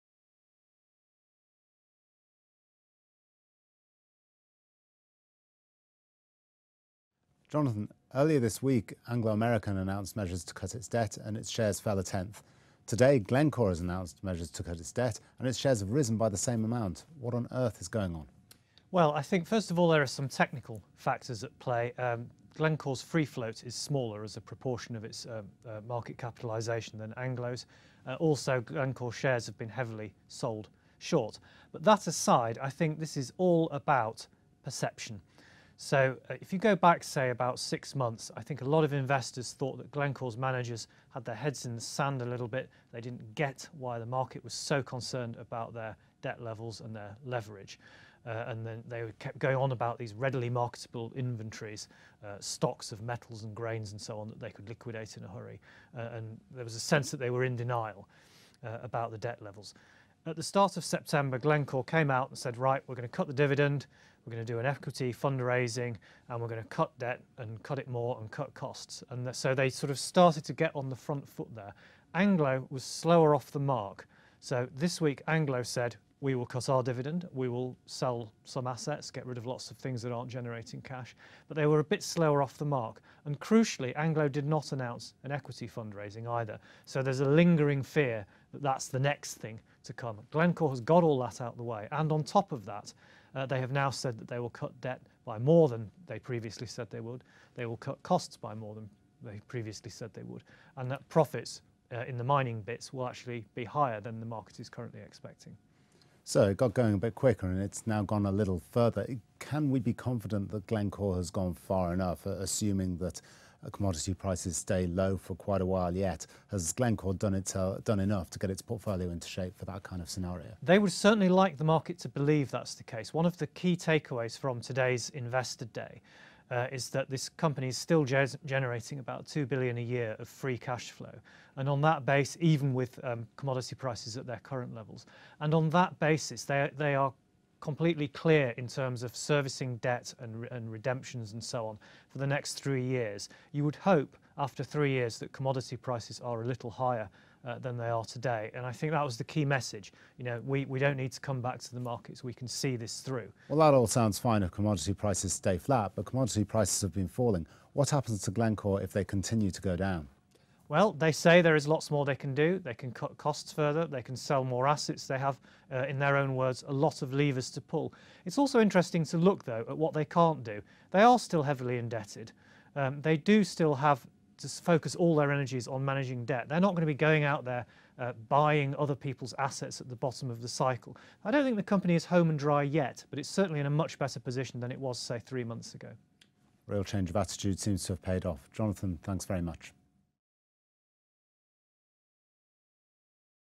Time for a Glencore turnround? | Lex
Shares in Glencore rose 14% after the miner raised its debt reduction target. Lex’s Oliver Ralph and Jonathan Eley discuss the group’s plans and whether they will be enough to stem concerns about its debt levels amid a collapse in commodity prices.